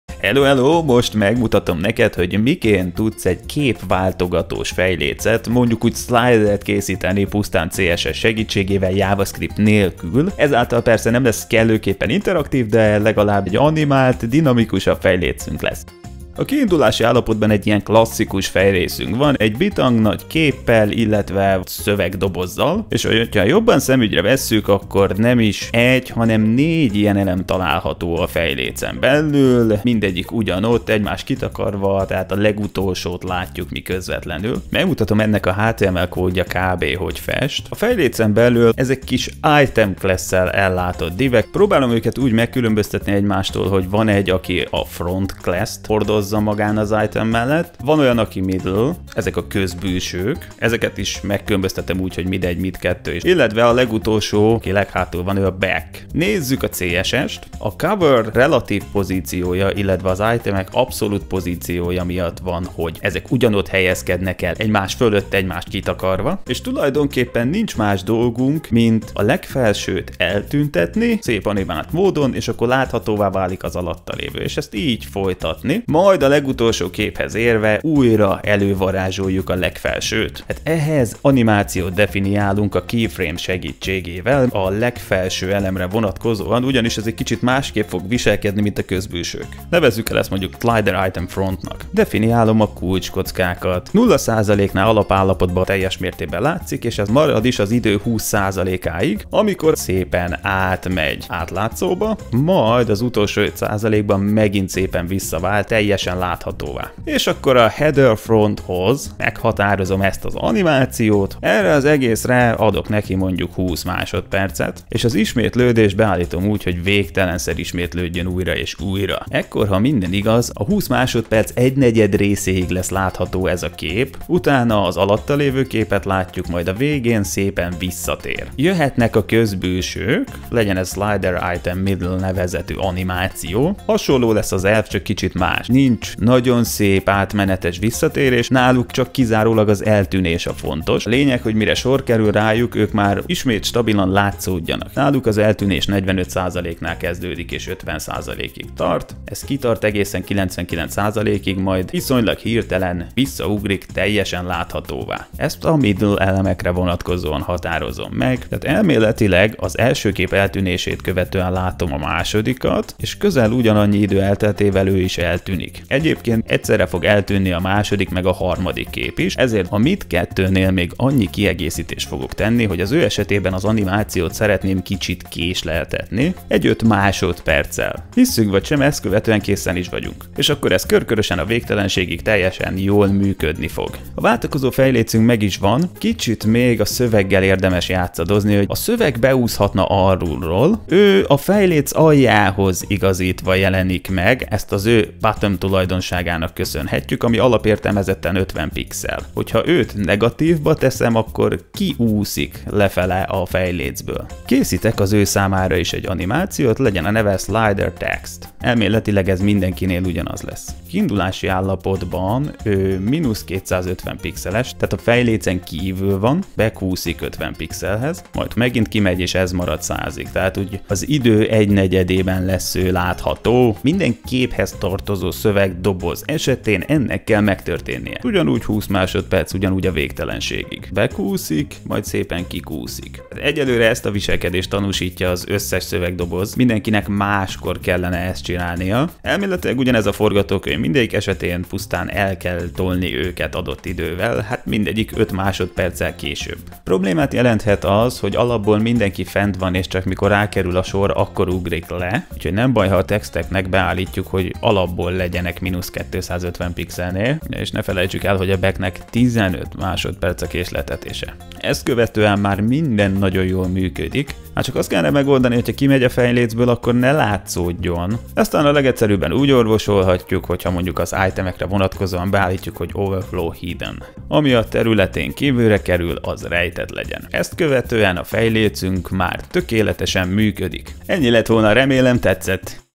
Váltakozó fejléc CSS-sel
Készítünk egy klassz animált képváltogató fejlécet (slider) CSS segítségével (JavaScript használata nélkül).